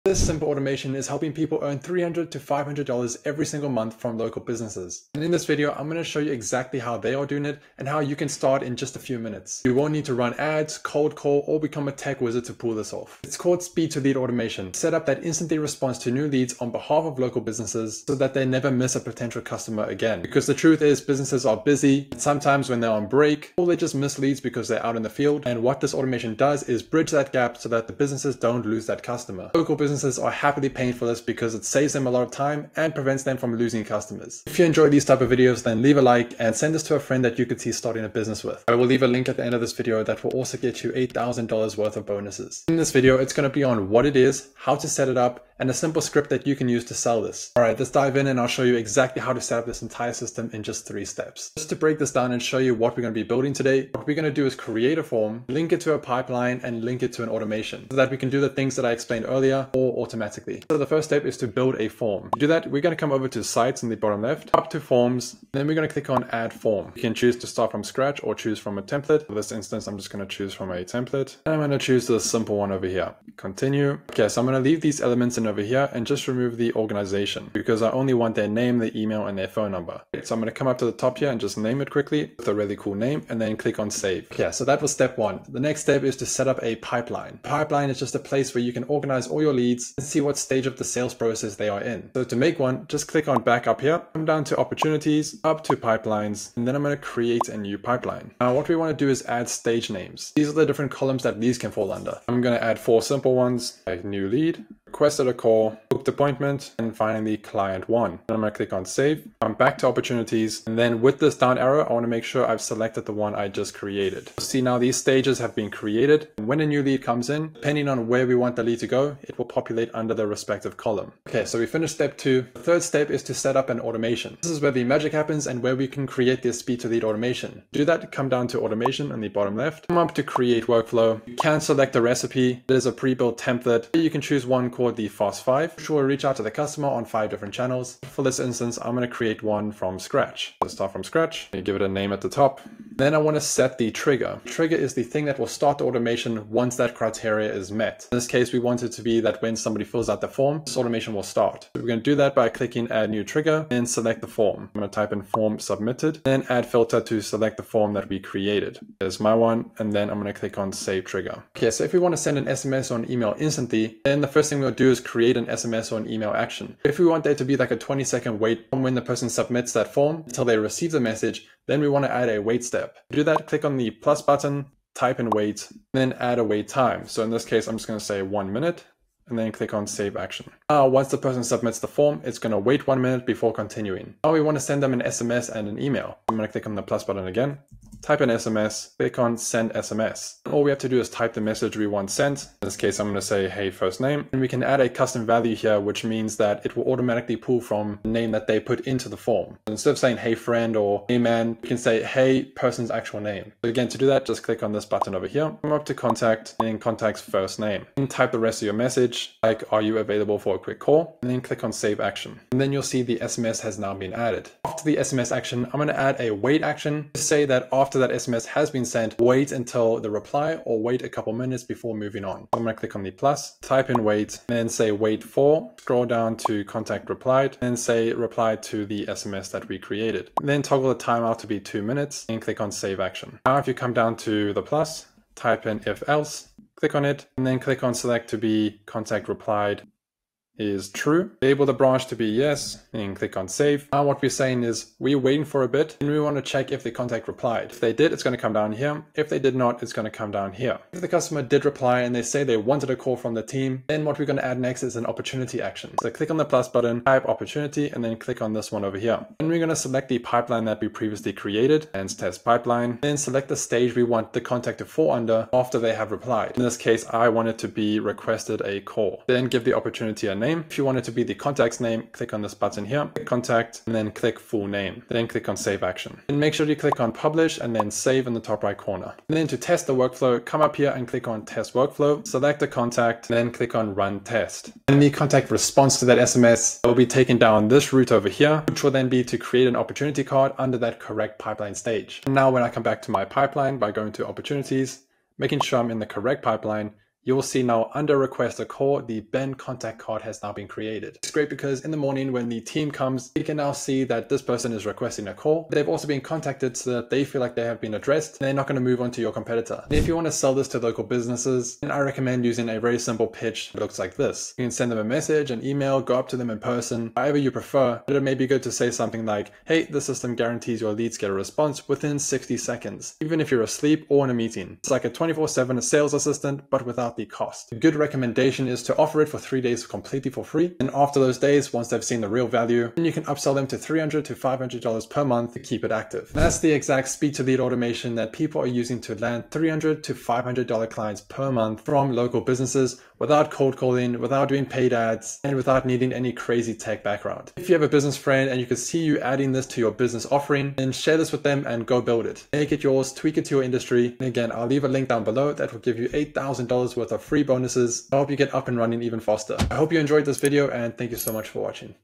How This GoHighLevel Feature Pays Me $500/Month
If you’ve already signed up and not received bonuses to your email, follow these steps:
1) Check your Spam Folder

Set up a “speed-to-lead” automation in GoHighLevel that replies to new leads instantly — and gets local businesses happily paying you $300–$500/month. Ben will walk you through the exact setup so you can launch it today.

00:00-Intro
00:47-Setup & Tutorial